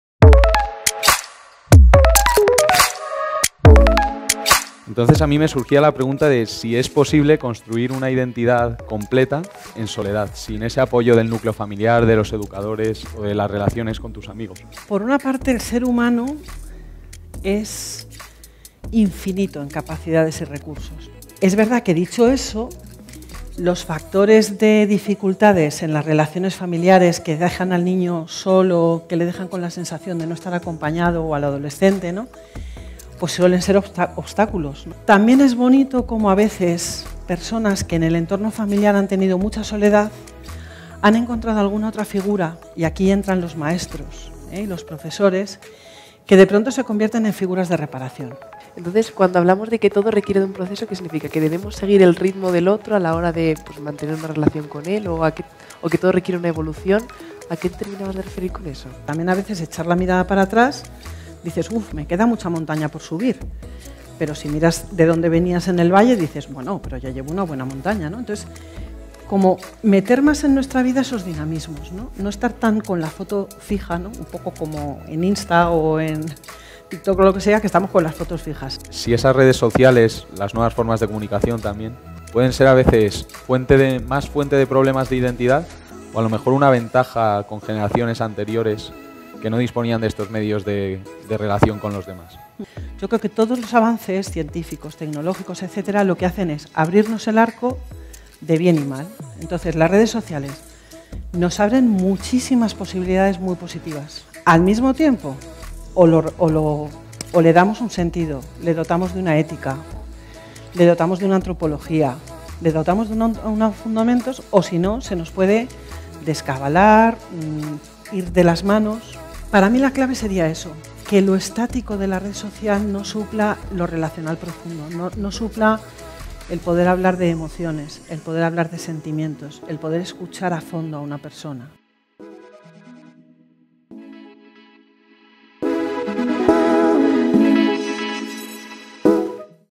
Preguntas Virginia Cagigal | La Educación que queremos
«Lo que construimos unos en los otros: Relaciones sociales e identidad»